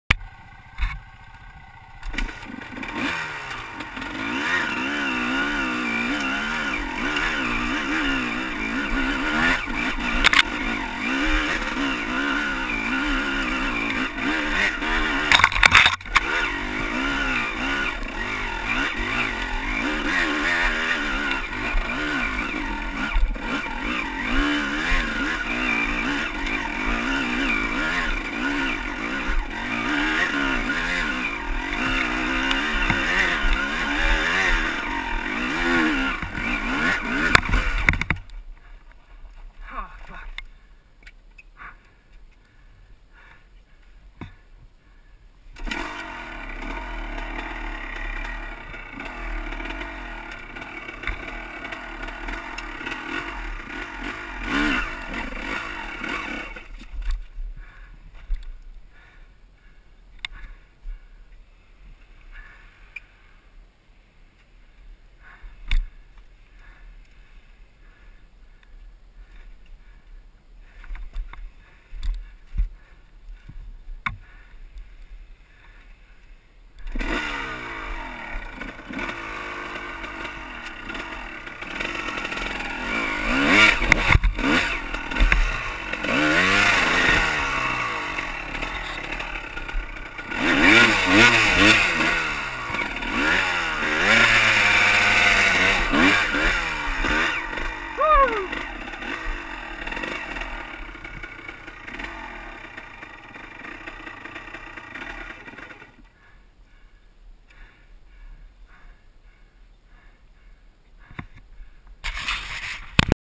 lefthand canyon spruce gulch ascent pt 2 the problem rock
pt 2 of the ascent on spruce gulch trail in lefthand canyon boulder colorado, and the infamous problem rock!